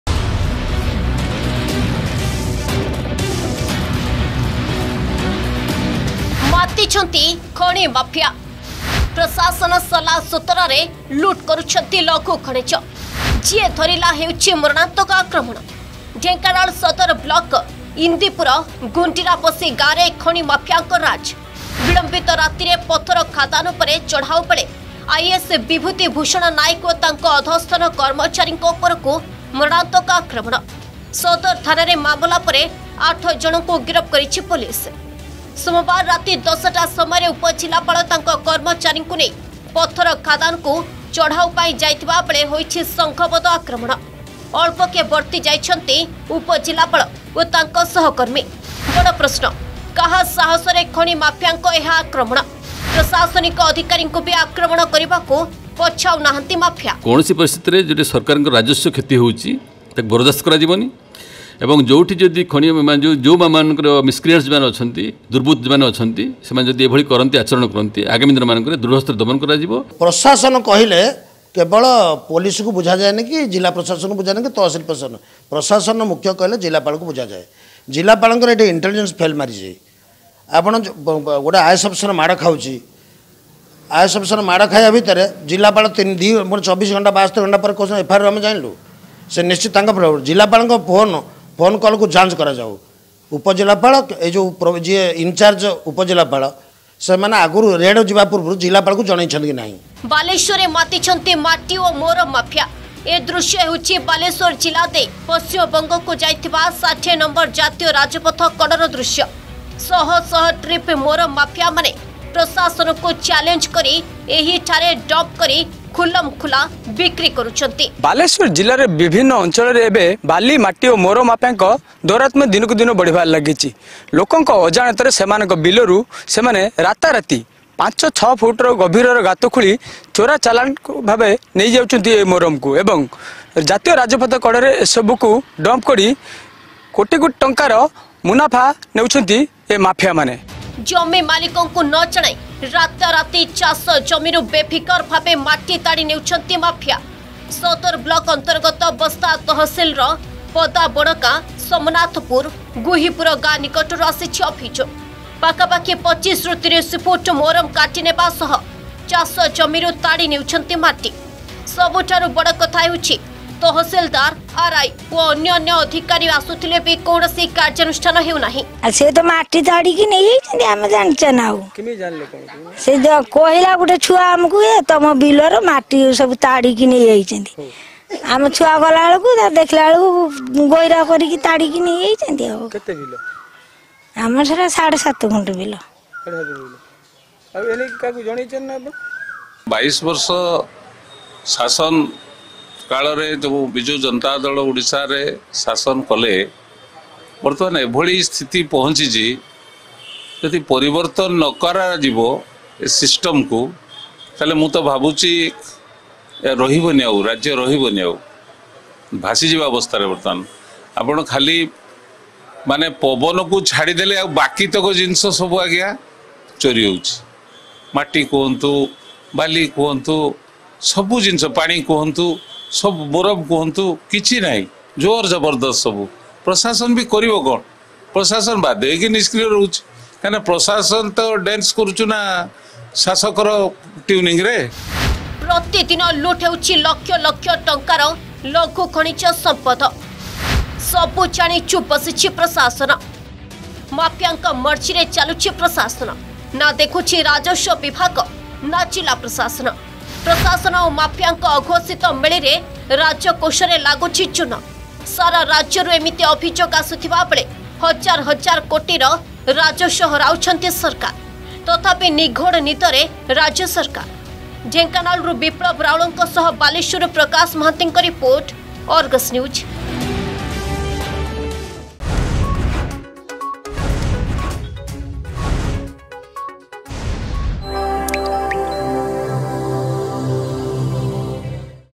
ମାତିଛନ୍ତି ଖଣି ମାଫିଆ
ରାଜ୍ୟରେ କୋଟି କୋଟି ଟଙ୍କାର ରାଜସ୍ବ ଲୁଟ୍ ।
 ଖଣି ମାଫିଆଙ୍କ ମର୍ଜିରେ ଚାଲୁଛି ପ୍ରଶାସନ ।
ପ୍ରଶାସନ ନାକତଳେ ପଥର, ବାଲି ଖାଦାନରୁ ଲୁଟ୍ ହେଉଛି ଲଘୁ ଖଣିଜ । ଢେଙ୍କାନାଳରେ ପ୍ରଶାସନିକ ଅଧିକାରୀଙ୍କ ଚଢାଉ ବେଳେ ମାଫିଆଙ୍କ ମରଣାନ୍ତକ ଆକ୍ରମଣ । ସେପଟେ ବାଲେଶ୍ବରରେ ଦିନ ଦ୍ବିପ୍ରହରରେ ଶହ ଶହ ଟ୍ରିପ୍ ମୋରମ ଚୋରା ଚାଲାଣ ହେଉଛି